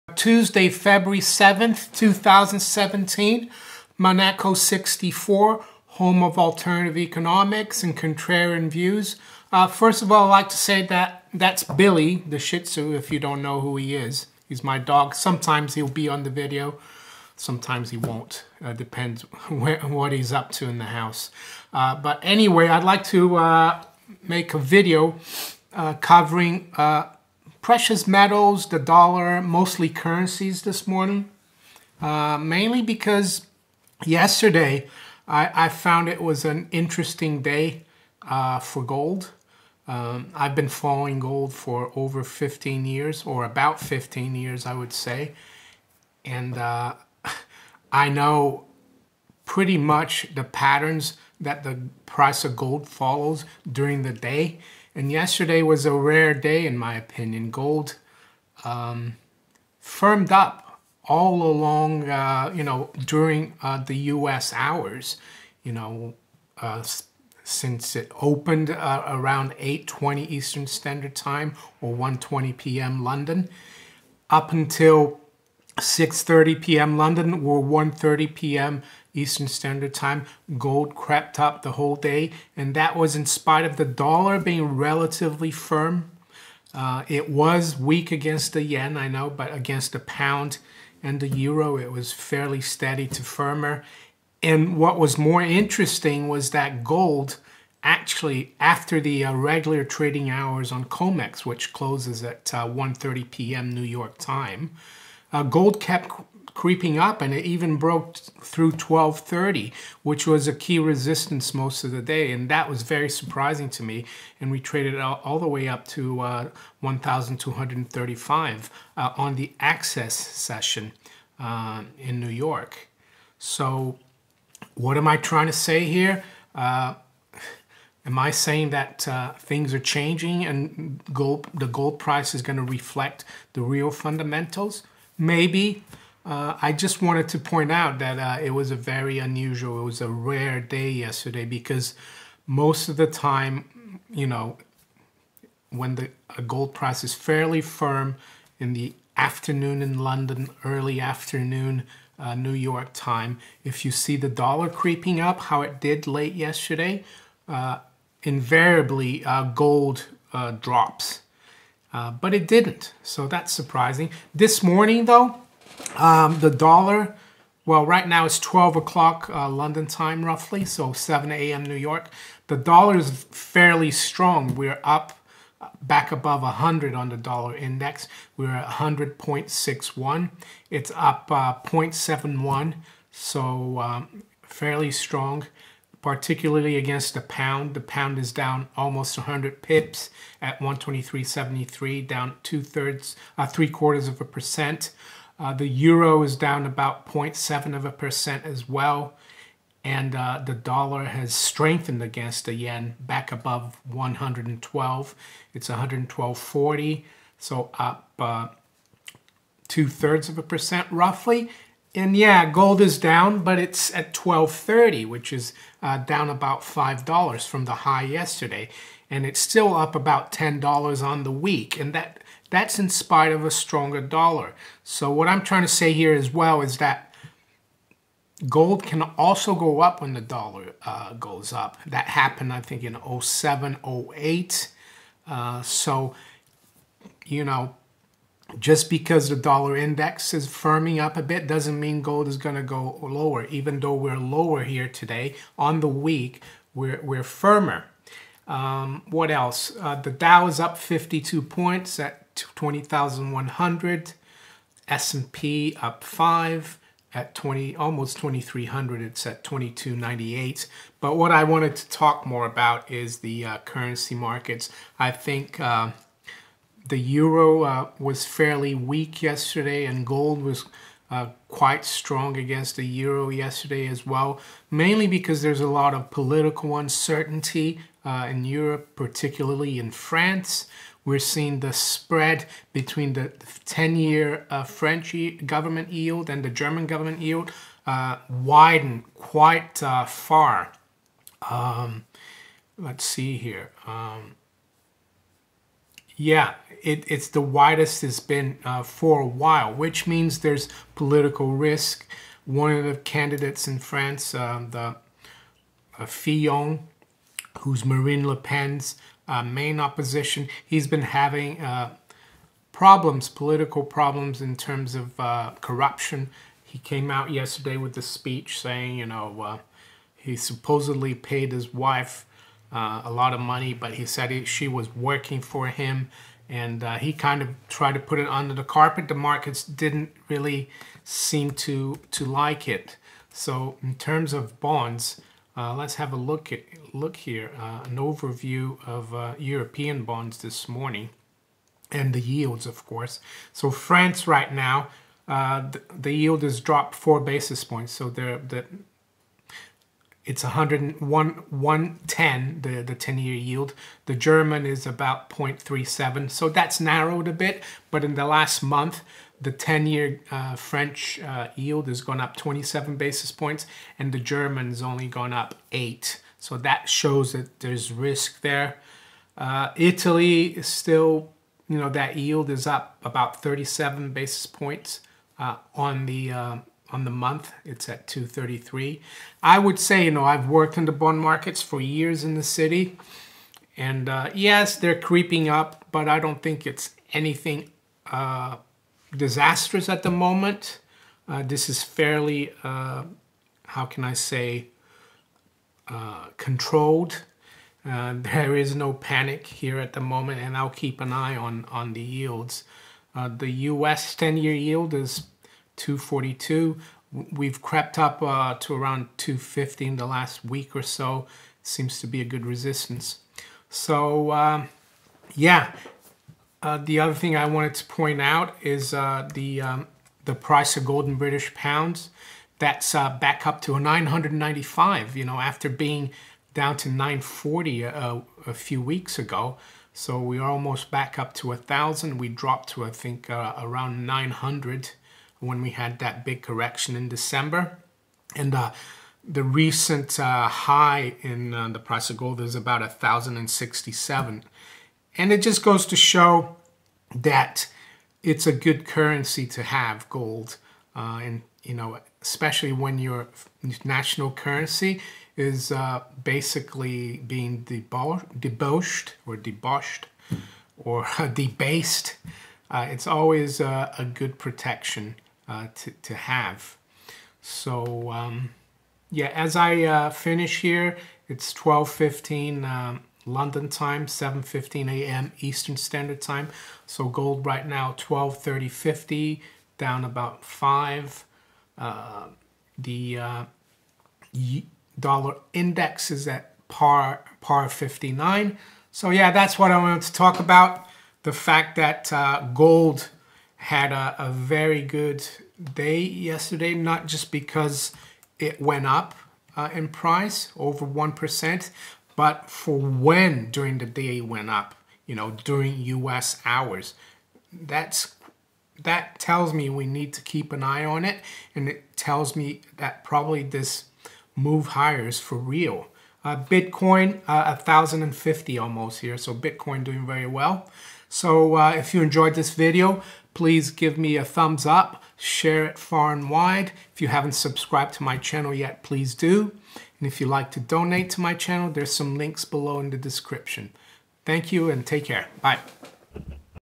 Encouraging Gold Price Action Despite Stronger Dollar.
In this report I cover the precious metals and currency markets. I look at the price action in gold on February 6th and 7th, 2017.